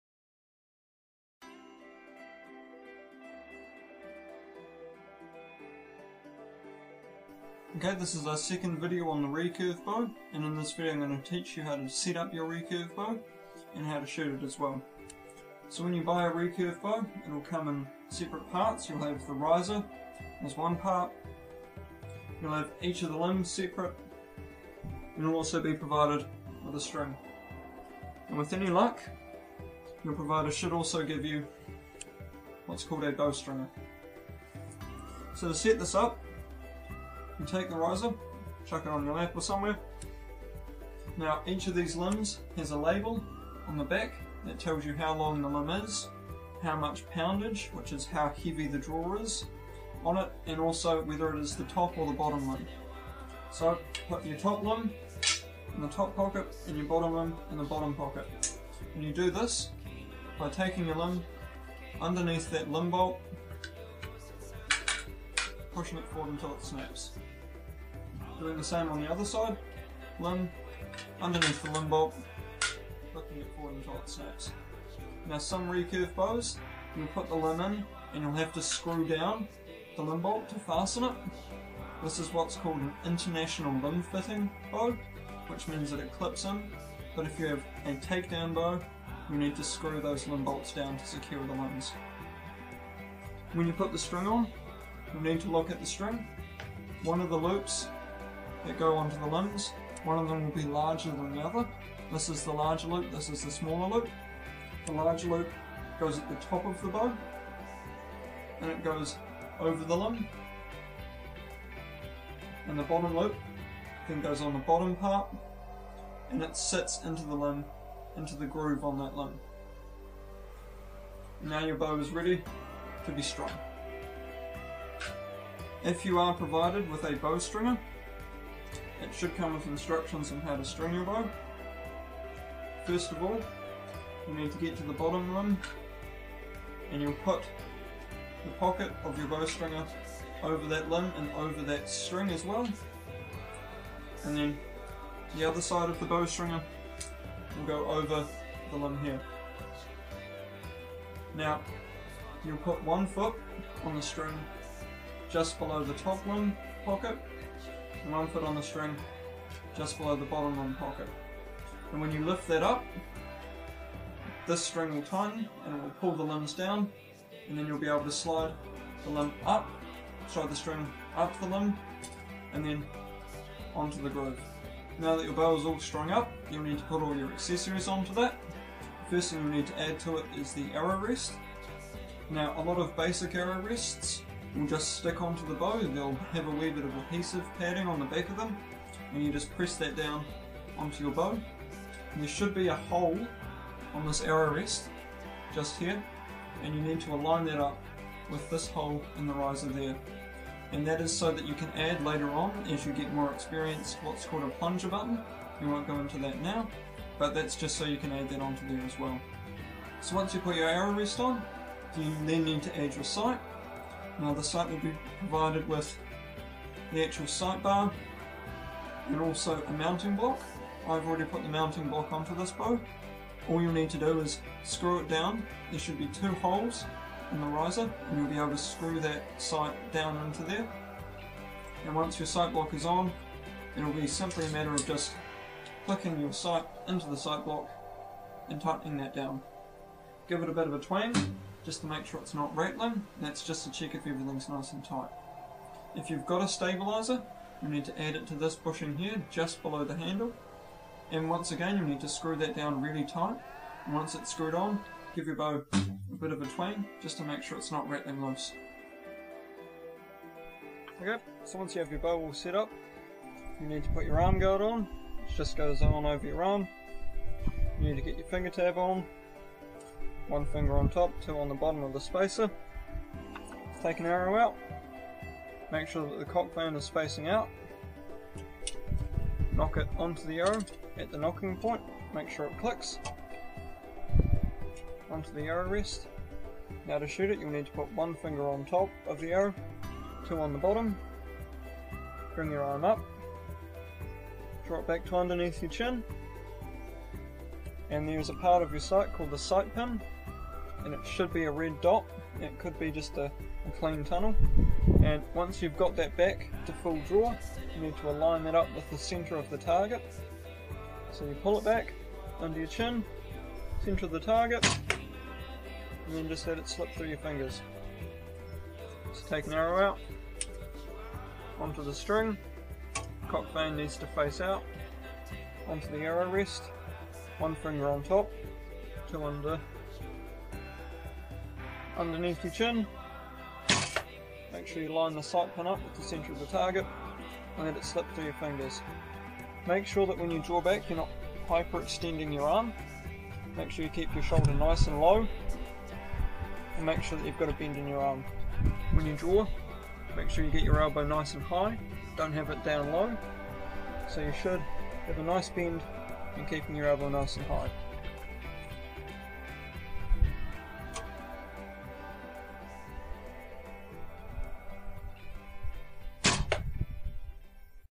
Recurve Bow - Video 2 - Assembling and Shooting the Recurve Bow
How to Set up your Recurve bow along with basic accessories, and how to shoot the Recurve with basic shooting form.